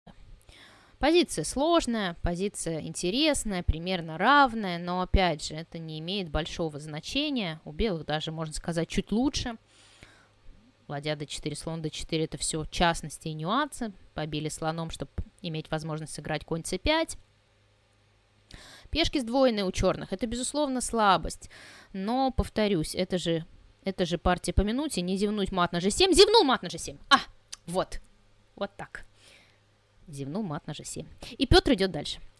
Мат - это самое главное
В полуфинале Титульного вторника противник семикратного чемпиона России гроссмейстера Петра Свидлера накопил больше времени на обдумывание и  активизировал фигуры, но... смотрите сами, что случилось дальше!

Следите вместе с нами за всеми этапами "Титульных вторников" и узнайте, кто получит прямую путевку в Чемпионат по скоростным шахматам.